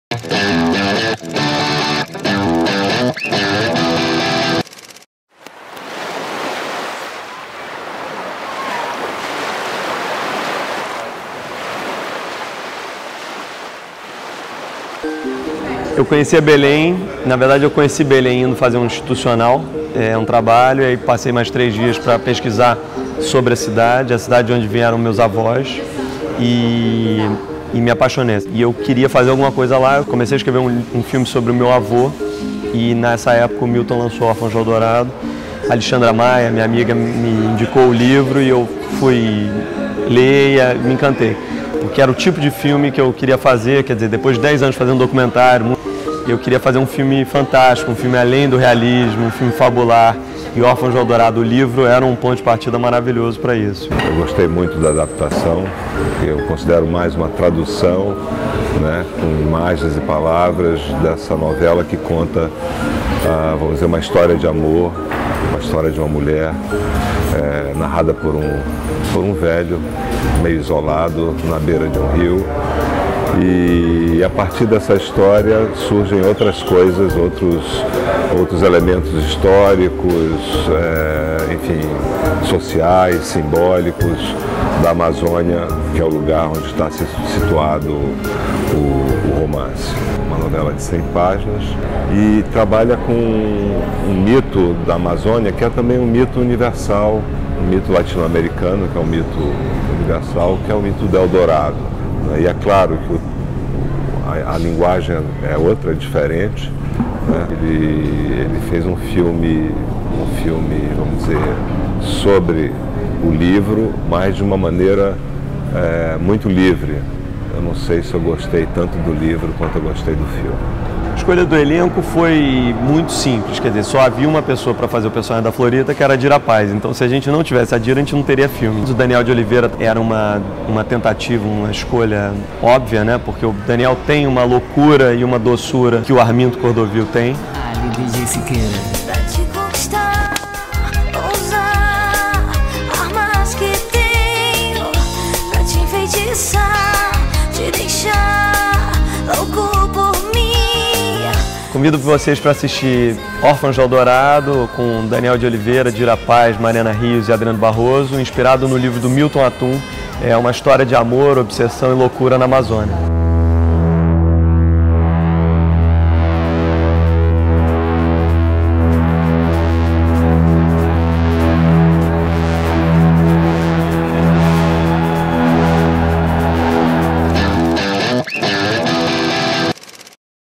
Curta! Cinema - Órfãos do Eldorado
Baseado no mais recente romance do escritor brasileiro Milton Hatoum, Órfãos do Eldorado é uma história de amor, obsessão e enlouquecimento de um homem na Amazônia. O diretor e o escritor comentam o lançamento do filme no Festival do Rio 2015.